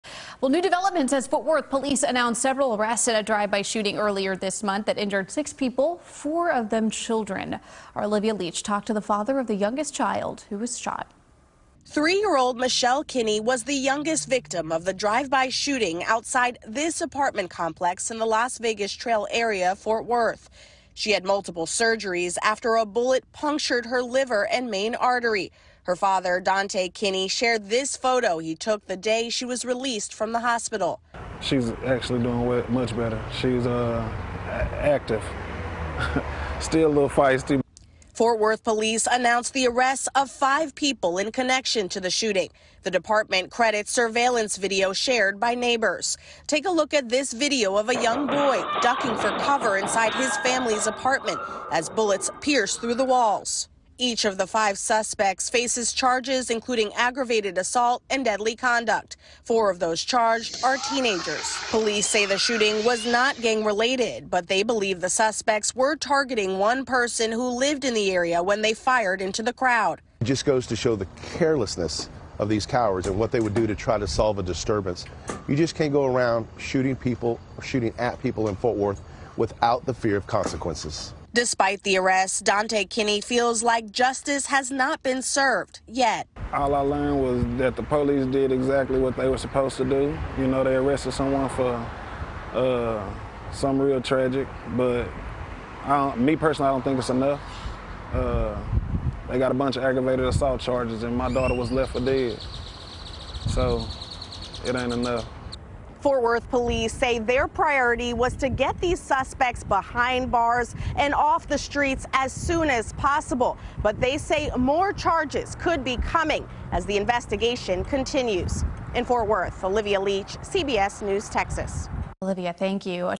Fort Worth police arrest five suspects in connection with drive-by shooting that injured 6, includin
Police arrest 5 suspects in connection with May 1 drive-by shooting on Las Vegas Trial in Fort Worth